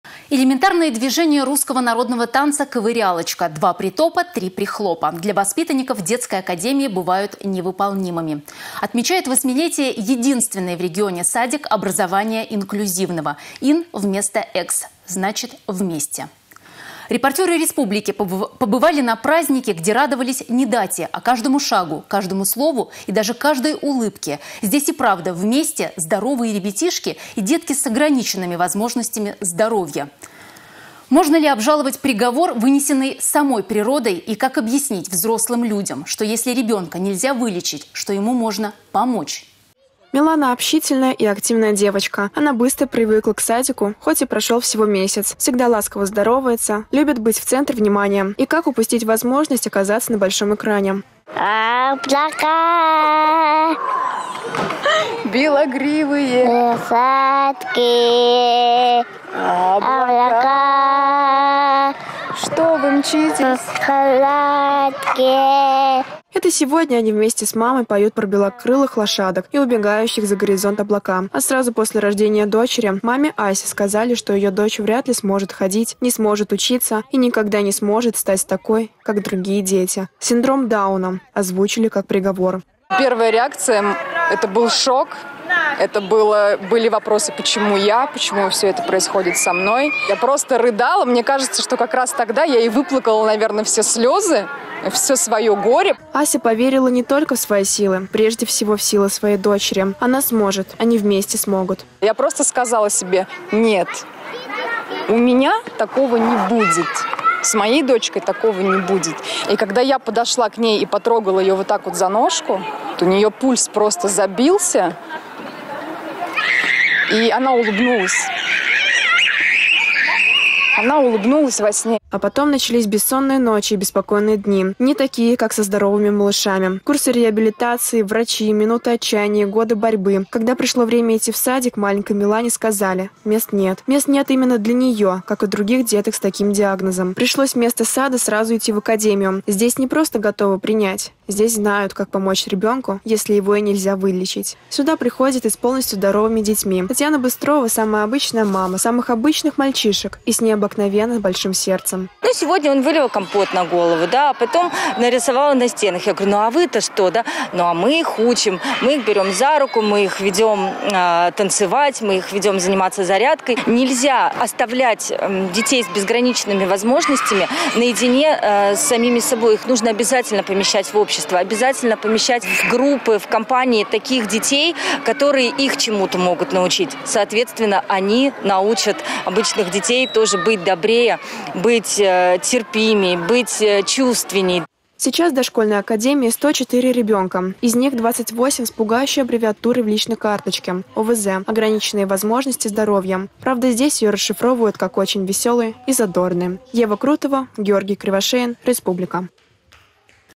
Единственный в Чувашии инклюзивный детский сад отмечает День рождения
Элементарные движения русского народного танца ковырялочка. «Два притопа, три прихлопа» для воспитанников Детской Академии бывают невыполнимыми. Отмечает восьмилетие единственный в регионе садик образования инклюзивного. Ин- вместо экс- значит вместе. Репортеры «Республики» побывали на празднике, где радовались не дате, а каждому шагу, каждому слову и даже каждой улыбке.